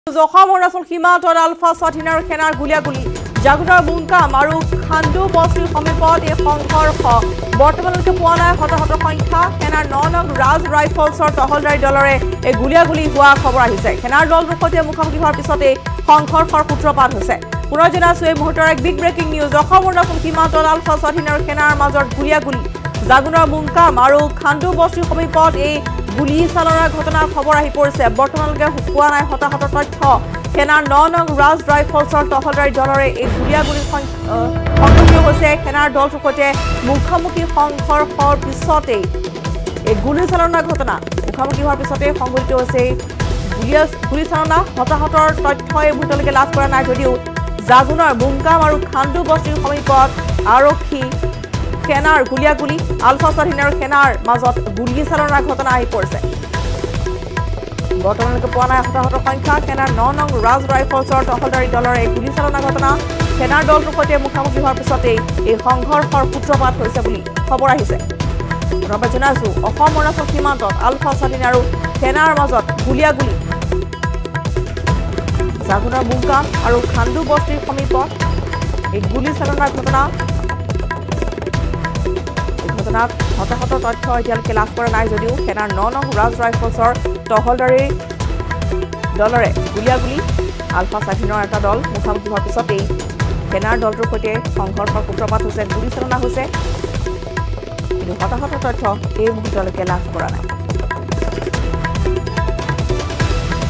Assam-Arunachal সীমান্তত ULFA(I) আৰু সেনাৰ গুলীয়াগুলী
অসম–অৰুণাচল সীমান্তৰ জাগুনৰ মুংকাম আৰু খান্দো বস্তিৰ সমীপত ভাৰতীয় সেনাৰ ৯নং ৰাজ ৰাইফলছ আৰু আলফা স্বাধীনৰ সদস্যৰ মাজত প্ৰচণ্ড গুলীয়াগুলী।